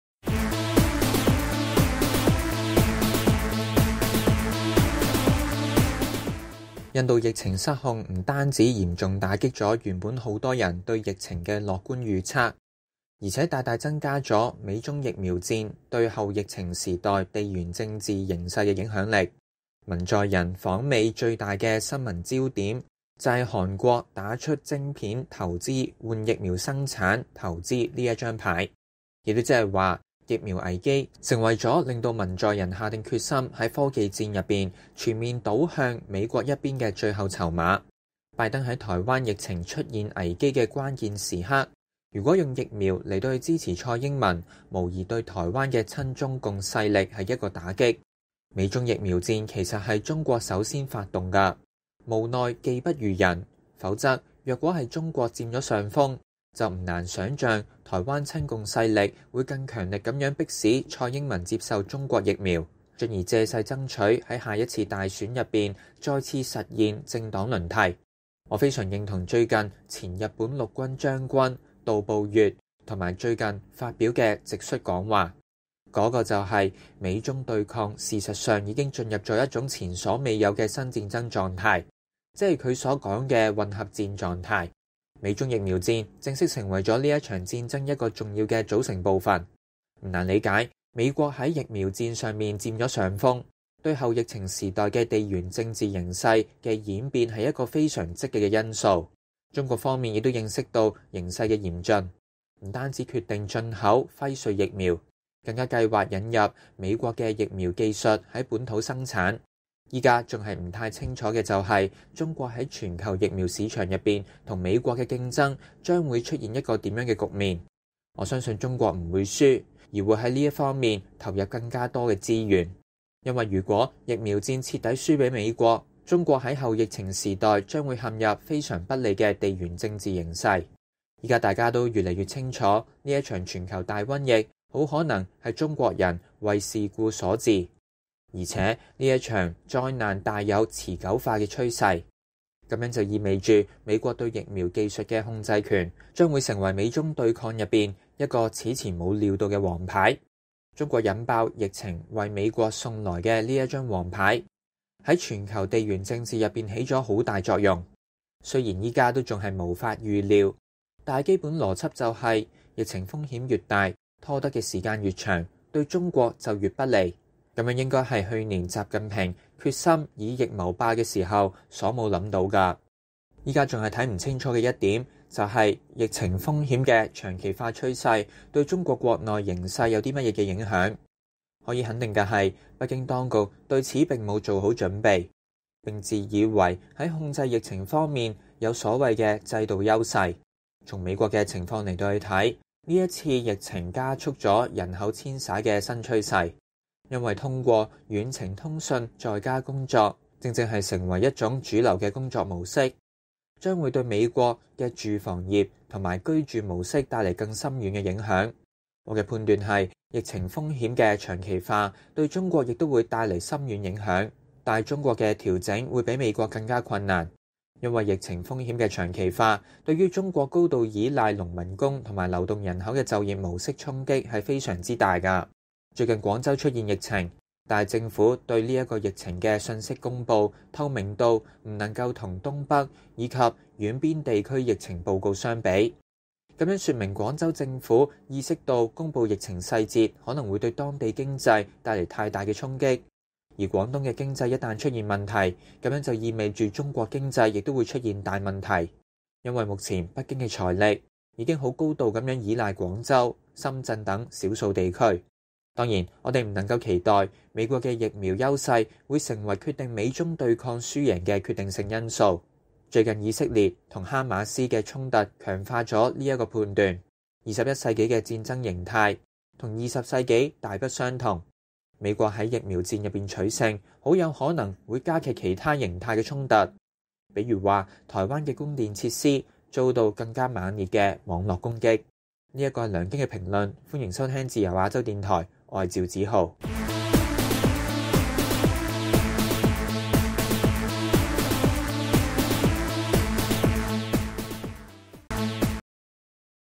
【梁京評論】美中疫苗戰與後疫情時代的地緣政治
印度疫情失控不僅嚴重打擊了原來許多人對疫情的樂觀預測，而且大大增加了美中疫苗戰對後疫情時代地緣政治形勢的影響力。文在寅訪美最大的新聞焦點，就是韓國打出了晶片投資換疫苗生產投資這張牌。也就是說，疫苗危機，成為讓文在寅下決心在科技戰中全面倒向美國一邊的最後砝碼。拜登在台灣疫情出現危機的關鍵時刻，如果用疫苗來支持蔡英文，無疑對台灣的親中共勢力是一個打擊。

（以上評論純屬作者個人觀點，並不代表本台立場。）